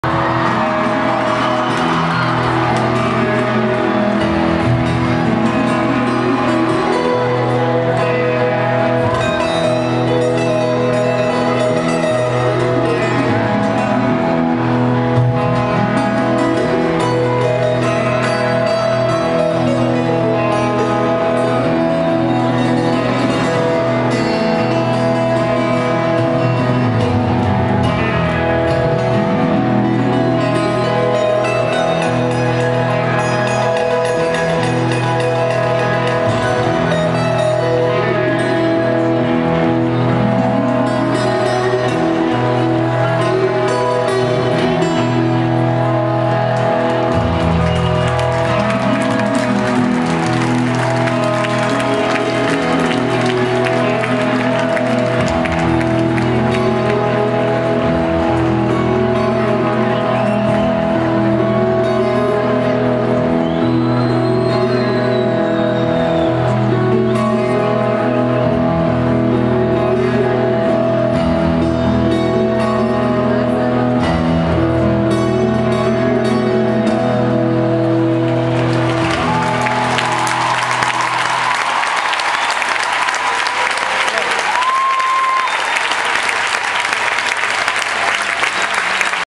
Negramaro a Bologna il 5.12.2008. Duetto di Lele e Giuliano
Concerto dei Negramaro a Bologna. Duetto di Lele "dalla lunga chioma" assieme a Giuliano.